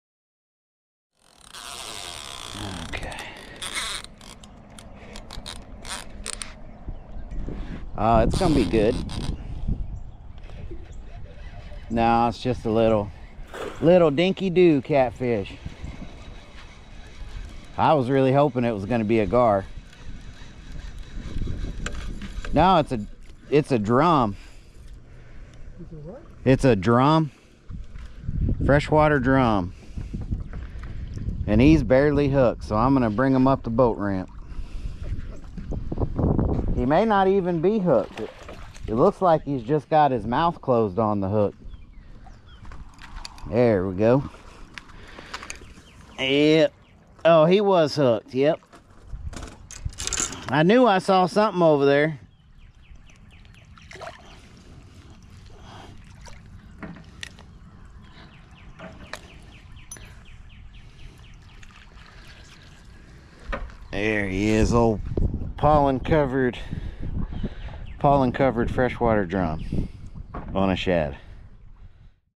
Freshwater Drum LOVE Fresh Shad Snacks
Today we fished the main lake from the bank. We used a cast net to catch some fresh bait. The shad were about 3" and have just began to spawn this year. I casted out and basically just let the shad sit on the bottom. The circle hook did its job and we picked up this cool looking freshwater drum.